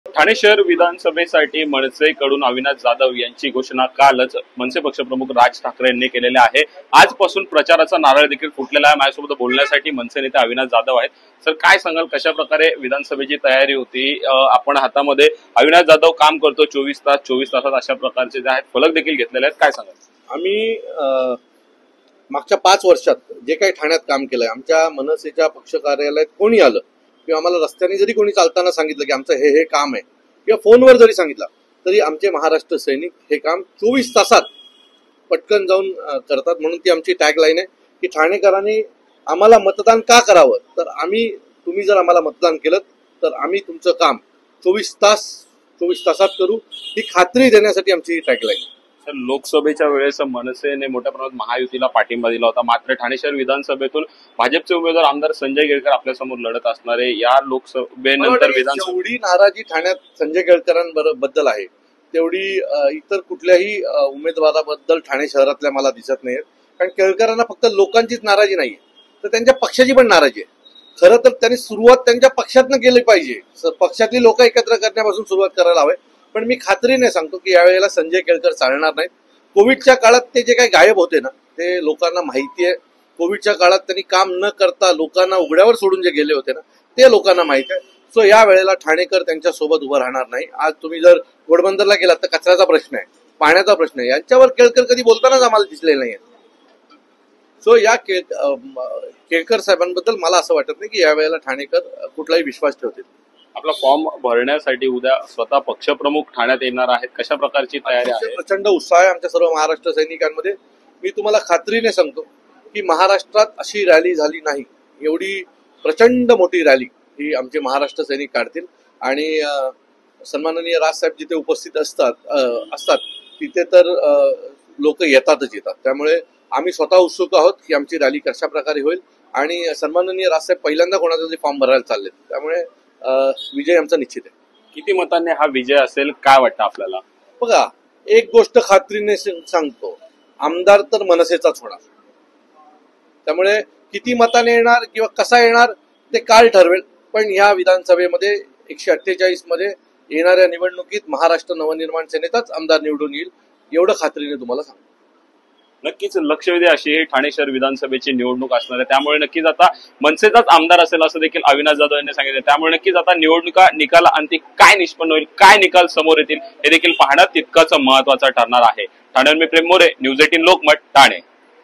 Avinash Jadhav Exclusive : अविनाश जाधवांना उमेदवारी जाहीर, प्रचाराला सुरुवात | Marathi News | N18V
Avinash Jadhav has been nominated by MNS for the Thane City Legislative Assembly Tomorrow, MNS party chief Raj Thackeray will come to Thane to file the nomination of Avinash Jadhav.